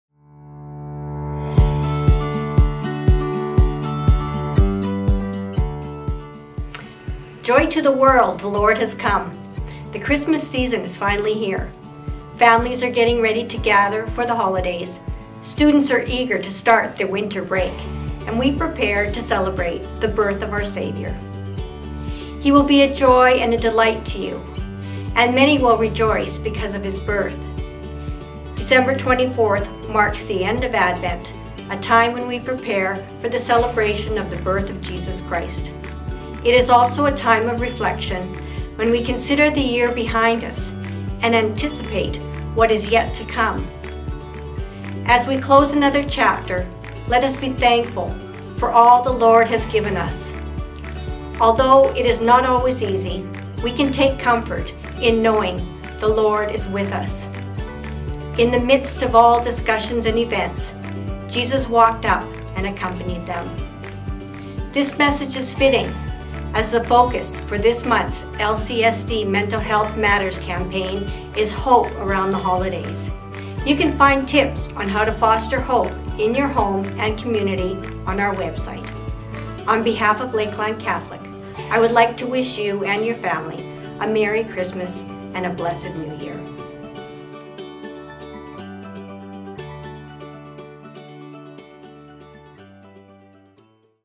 December 2022 - Trustee Corner
Trustee Diane Bauer shares December's Trustee Corner.

lrcssd.ca